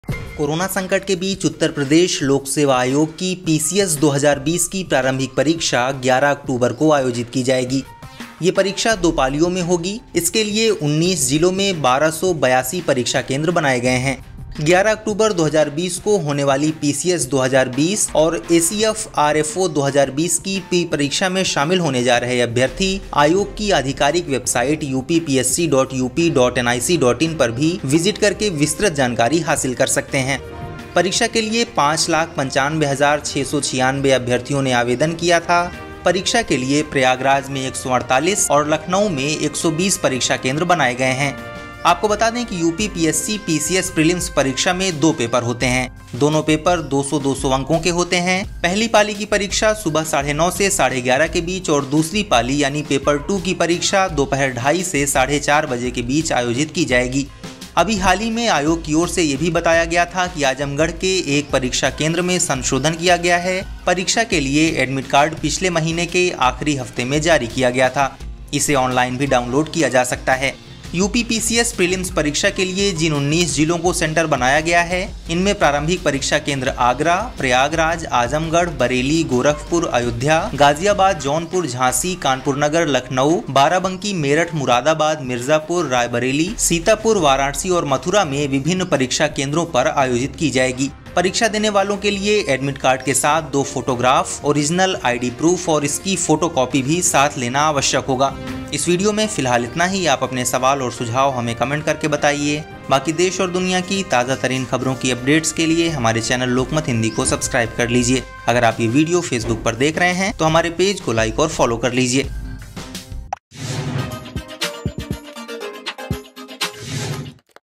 UPPSC PCS Prelims 2020: 11 अक्टूबर को होगी परीक्षा, 19 जिलों में बनाए गए 1282 परीक्षा केंद्र
कोरोना संकट के बीच उत्तर प्रदेश लोक सेवा आयोग (UPPSC) की PCS 2020 की प्रारंभिक परीक्षा 11 अक्टूबर को आयोजित की जाएगी। ये परीक्षा दो पालियों में आयोजित की जाएगी। इसके लिए 19 जिलों में 1282 परीक्षा केंद्र बनाए गए हैं।
11 अक्टूबर 2020 को होने वाली पीसीएस 2020 और एसीएफ/ आरएफओ 2020 की प्री-परीक्षा में शामिल होने जा रहे अभ्यर्थी आयोग की आधिकारिक वेबसाइट uppsc.up.nic.in पर भी विजिट करके विस्तृत जानकारी हासिल कर सकते हैं।
परीक्षा के लिए 5 लाख 95 हजार 696 अभ्यर्थियों ने आवेदन किया था। परीक्षा के लिए प्रयागराज में 148 और लखनऊ में 120 परीक्षा केंद्र बनाए गए हैं।